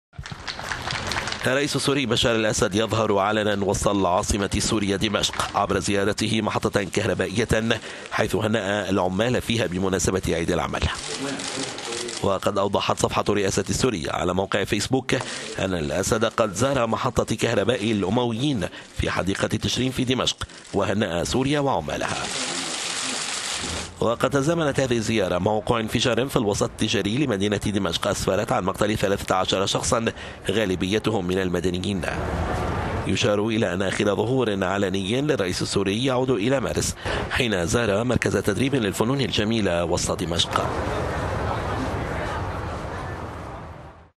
بشار الأسد يظهر علنا ويهنأ العمال السوريين
الرئيس السوري بشار الأسد يظهر علنا وسط دمشق عبر زيارته محطة كهربائية حيث هنأ العمال فيها مناسبة عيد العمل. وقد اوضحت صفحة الرئاسة السورية على موقع فيسبوك أن الأسد قد زار محطة كهرباء الأمويين في حديقة تشرين في دمشق وهنأ سورية وعمالها .وقد تزامنت هذه الزيارة مع وقوع انفجار في الوسط التجاري لمدينة دمشق اسفرت عن مقتل ثلاثة عشر شخصا غالبينتهم من المدنيين .يشار الى أن اخر ظهور علني للرئيس السوري إلى مارس حين زار مركز تدريب للفنون الجميلة في وسط دمشق.